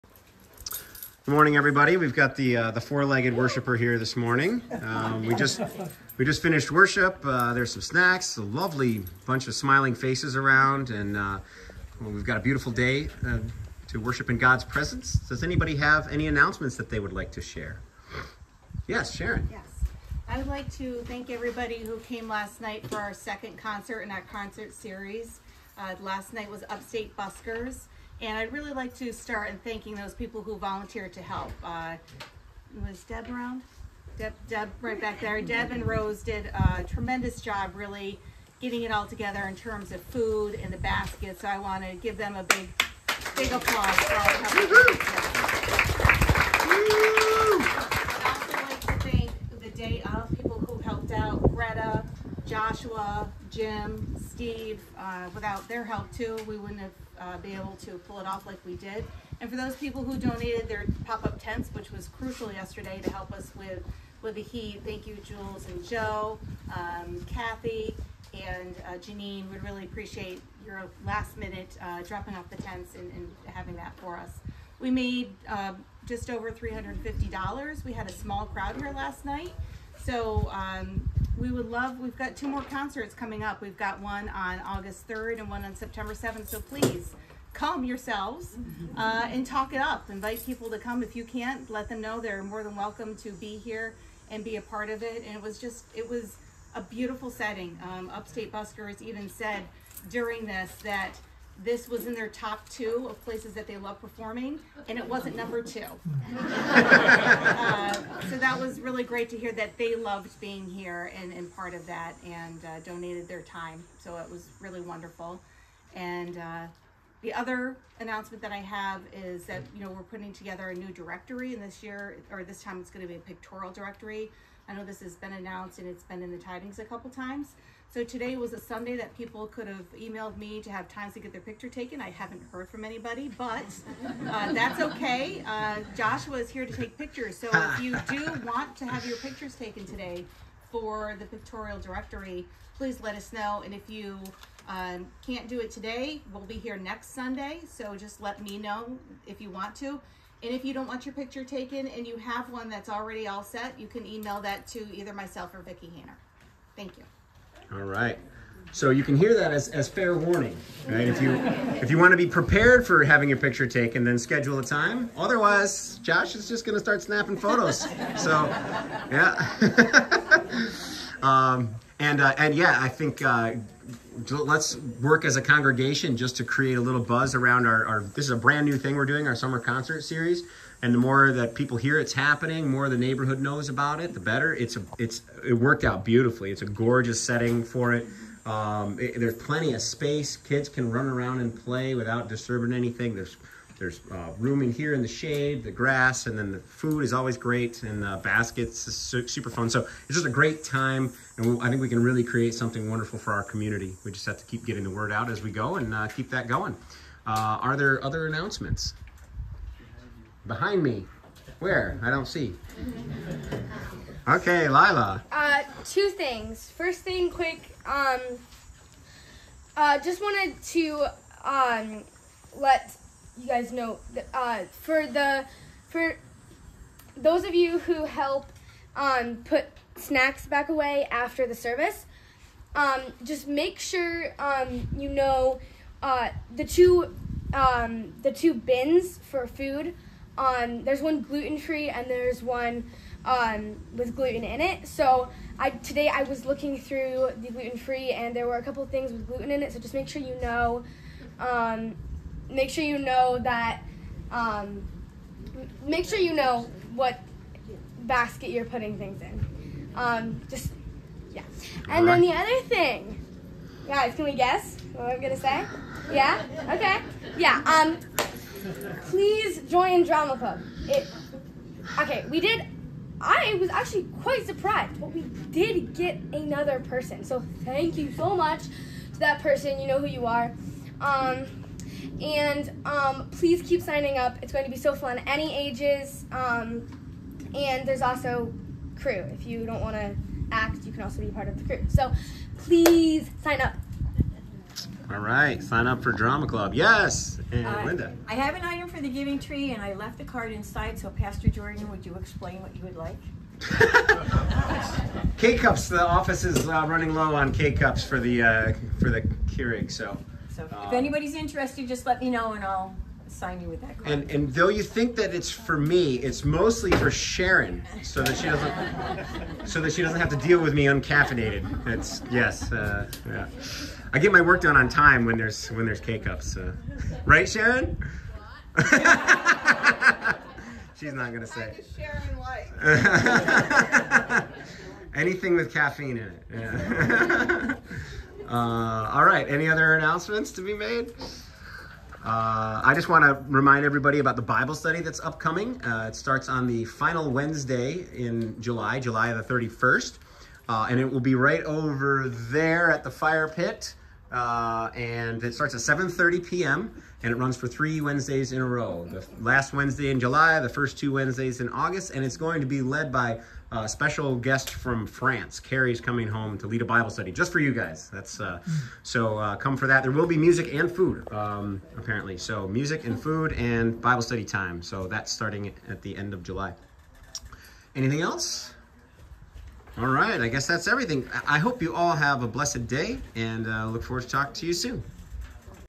WGRC - Sunday Announcements - 7.14.24
This week's Sunday Announcements following worship in the amphitheater.